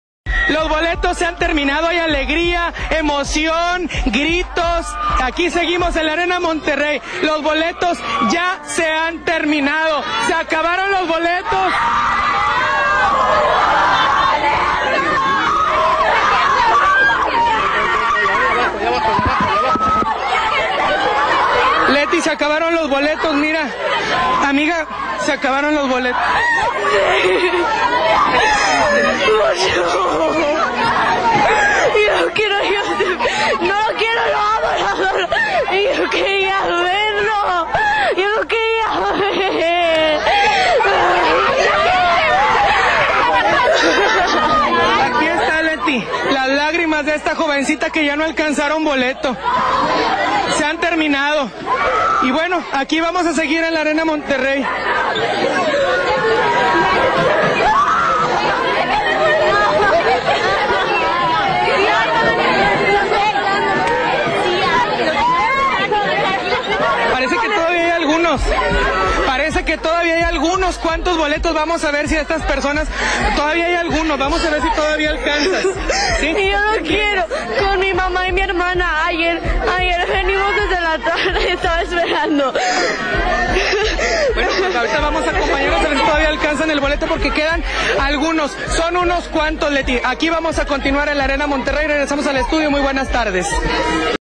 Трагедия века. Закончились билеты на Джастина Бибера
Когда в кассах закончились билеты, у маленьких девочек началась истерика и горькое сожаление по этому поводу. Пичалька :(